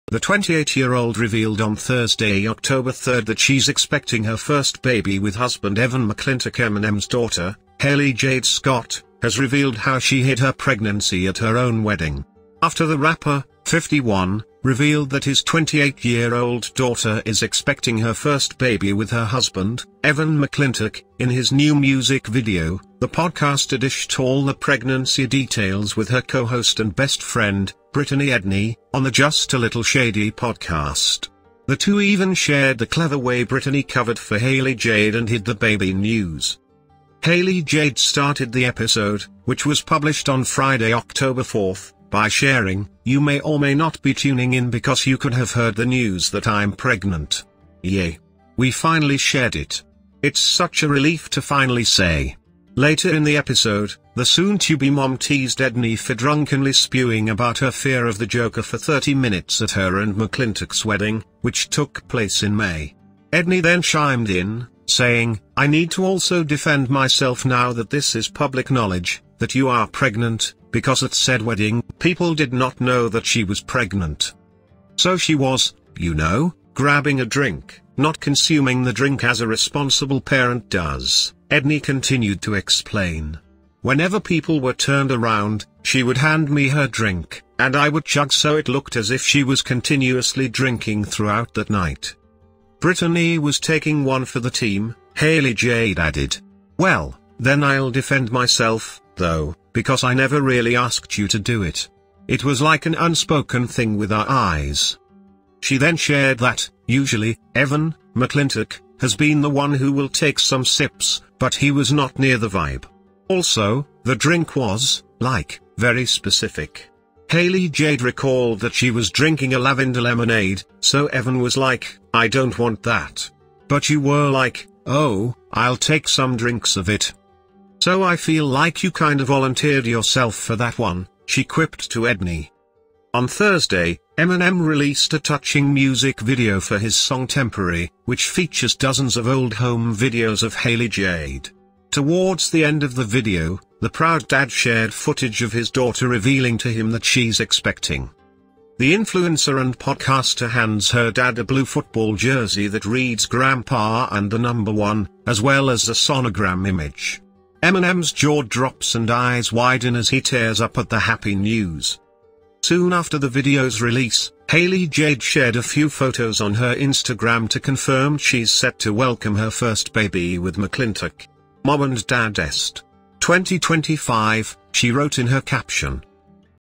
Hailie Jade Reveals How She Hid Her Pregnancy at Her Own Wedding with Help from BFF Brittany Ednie
In this video, Hailie Jade opens up about keeping her pregnancy a secret at her wedding with the help of her best friend, Brittany Ednie. Watch as she shares her experience and how she managed to keep such a big secret.